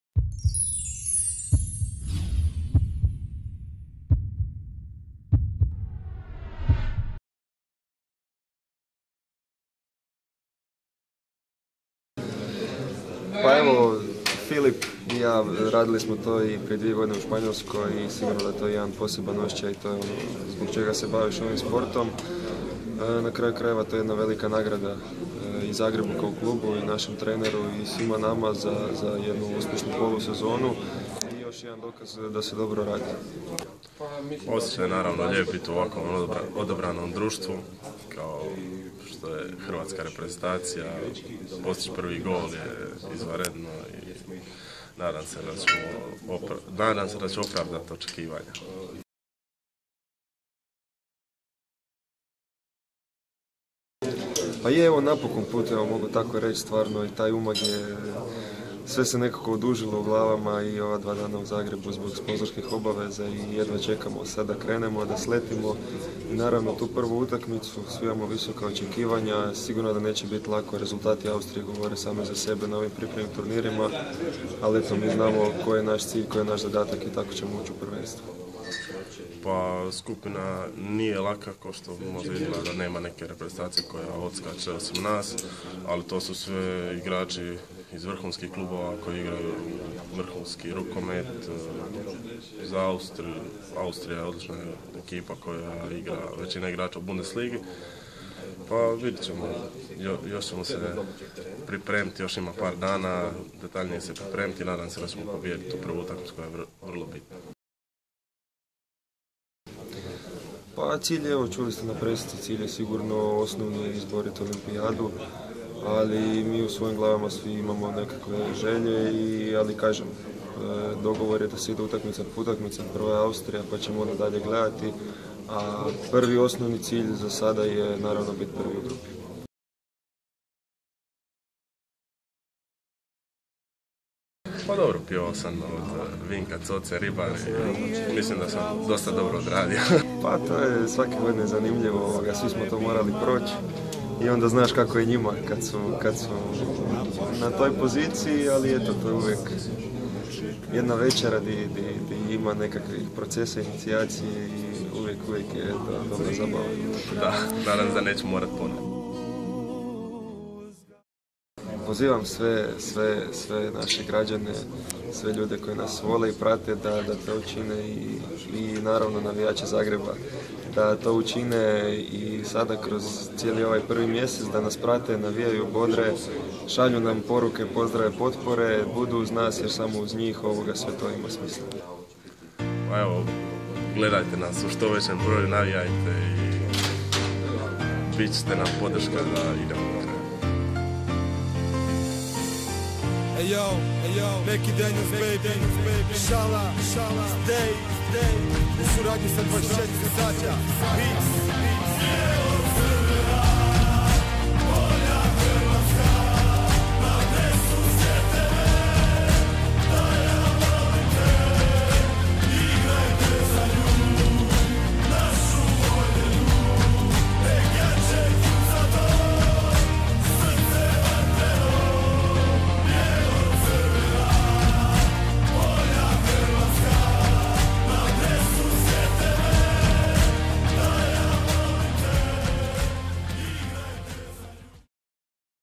Luka Stepančić i Ilija Brozović uoči Svjetskog prvenstva - Qatar2015
Zagrebaši Luka Stepančić i Ilija Brozović podijelili su svoja razmišljanja o predstojećem Svjetskom prvenstvu u rukometu koje će se održati u Kataru od 15.02. do 2.02. ove godine.

Sretno svim našim Kaubojima i učinite nas ponosnima kao i uvijek!

IZNAD SVIH...HRVATSKA!!!